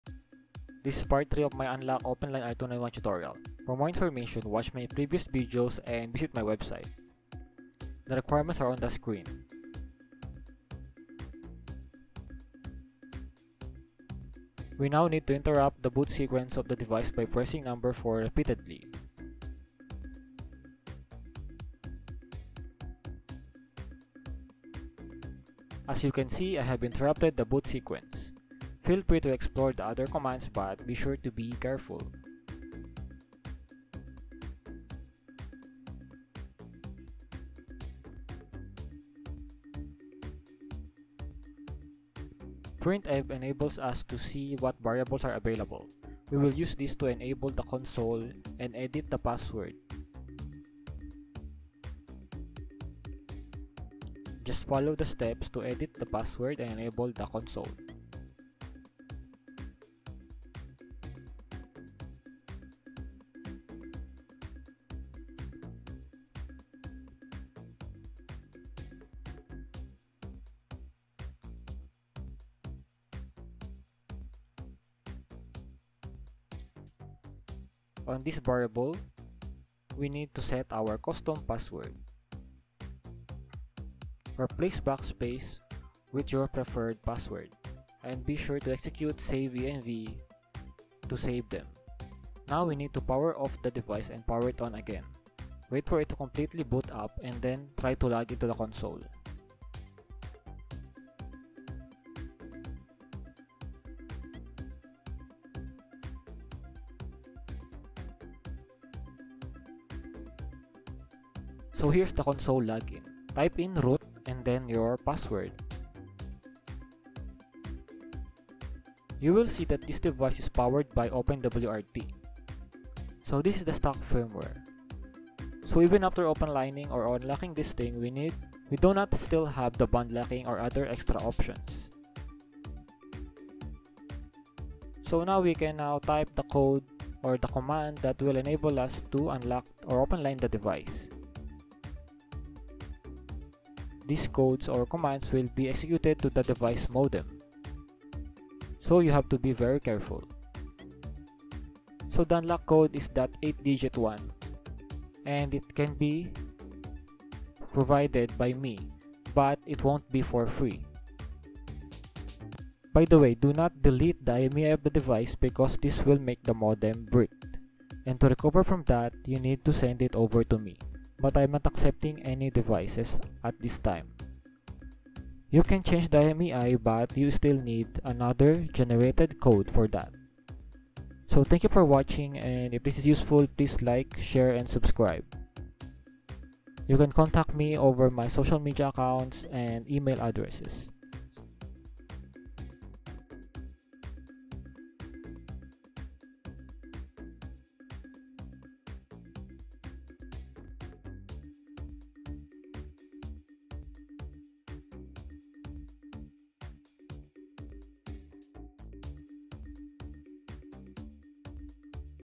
Part 3 How to Unlock/Openline Smartbro R291 using USB Serial
Smart Bro R291 CAT6 LTE 4G WiFi Router

Unlock/Openline:

ubus call version set_atcmd_info '{"atcmd":"AT+CLCK=PN,0,12345678,16"}'

Change 12345678 to the unlock code provided by me.

Change imei:
On stock firmware, bricking the module is very likely!!!

ubus call version set_atcmd_info '{"atcmd":"AT*PROD=2"}'
ubus call version set_atcmd_info '{"atcmd":"AT*MRD_IMEI=D"}'
ubus call version set_atcmd_info '{"atcmd":"AT*MRD_IMEI=W,0101,01NOV2012,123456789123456"}'
ubus call version set_atcmd_info '{"atcmd":"AT*PROD=0"}'

Change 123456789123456 to your preferred IMEI.
DO NOT DELETE IMEI, IF YOU DO,THE MODULE WILL BE BRICKED!
HIGH PROBABILITY OF MODEM BRICK IF YOU CHANGE IMEI.
I DO NOT RECOMMEND THIS AT THIS TIME.

No bandlocking interface, this is stock firmware factory unlocking.